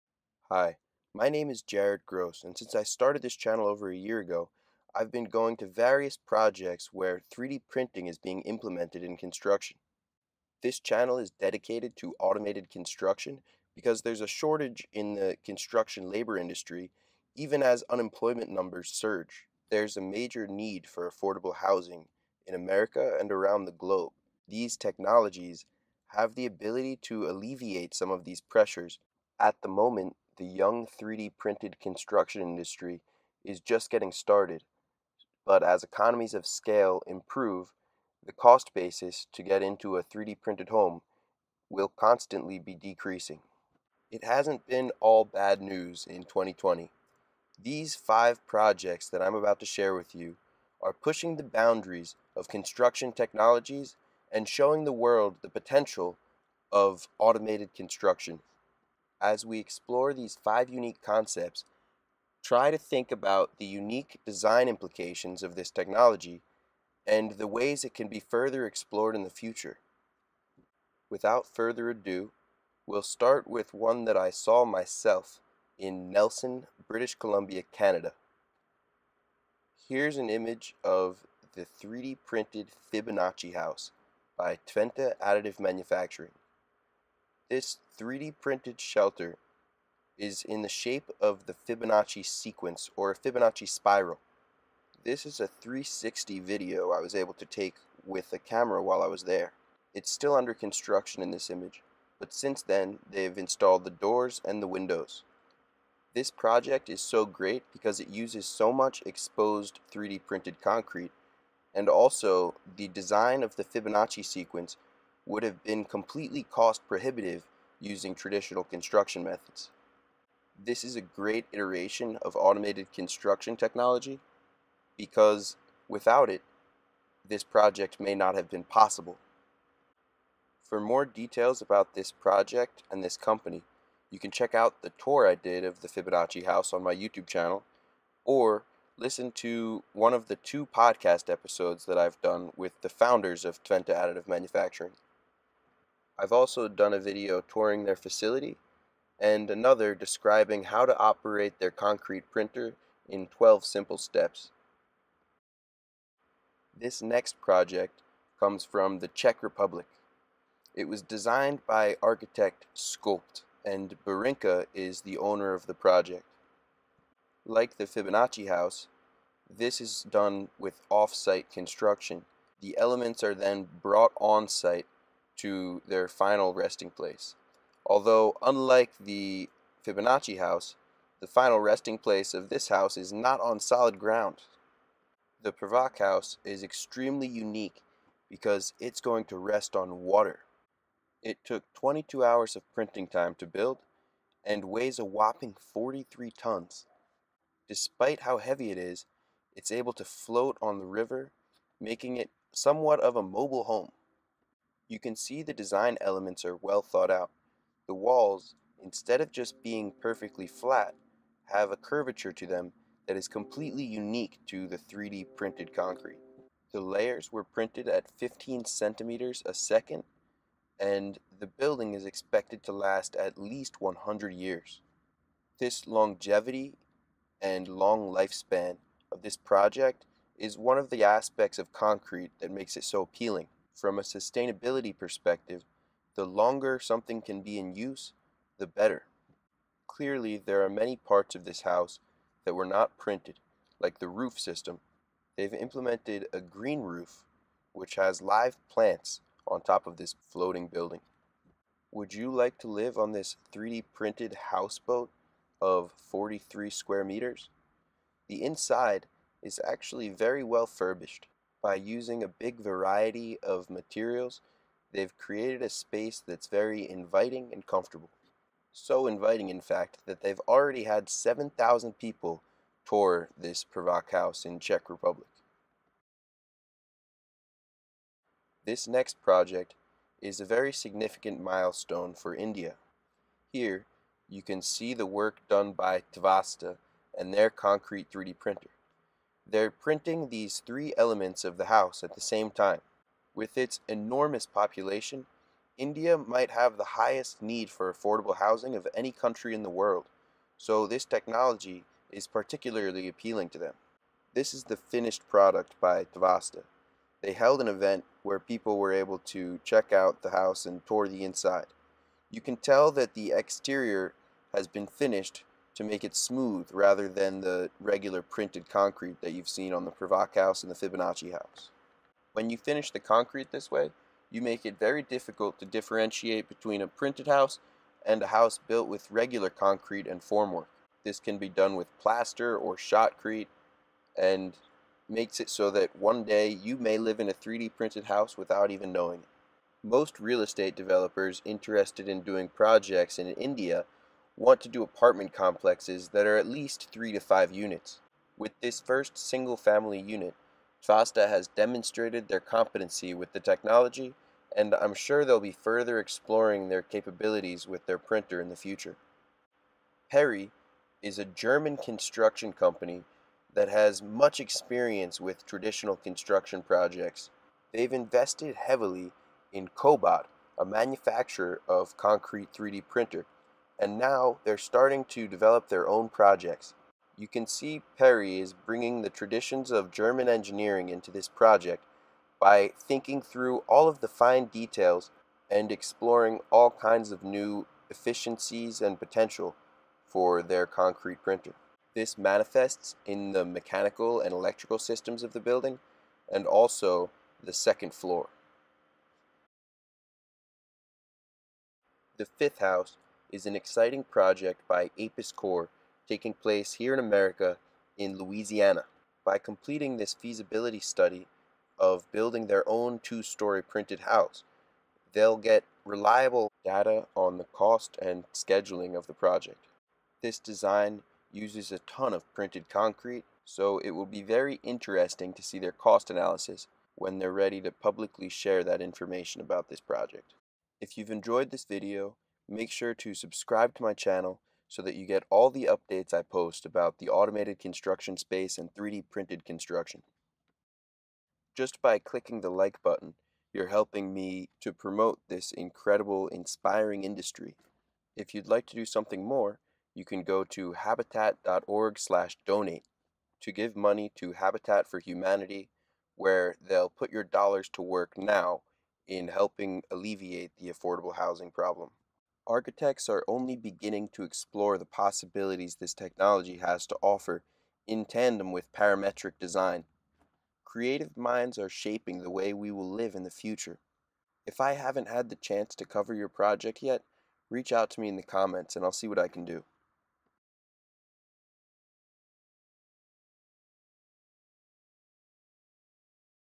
Top 5 3D Printed Houses of 2020
These new ambitious projects are pushing the boundaries in automated construction. Many great homes were printed before 2020 but they have been excluded from this list.

Sign up for the "How to 3D Print a House" course available now at the beta pricing!

(No Particular Order)

Chapters
0:00 Intro
1:18 Fibonacci House by Twente Additive Manufacturing
2:36 Prvok by Burinka
4:38 Tvasta House
5:59 2 Story Peri House
6:41 2 Story Apis Cor House